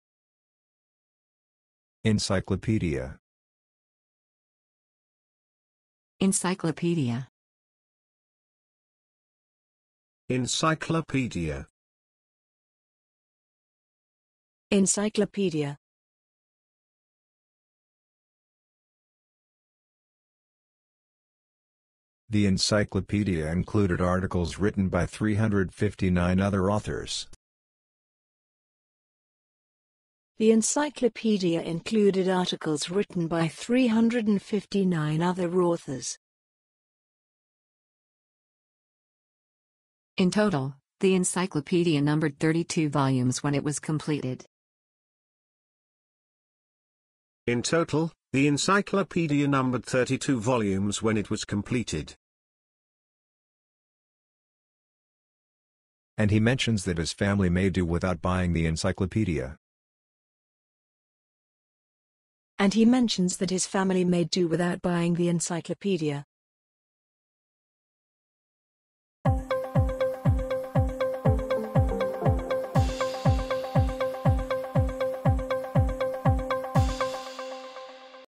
ENCYCLOPEDIA pronunciation | Improve your language with bab.la
Improve your spoken English by listening to ENCYCLOPEDIA pronounced by different speakers – and in example sentences too. Learn and love languages with bab.la.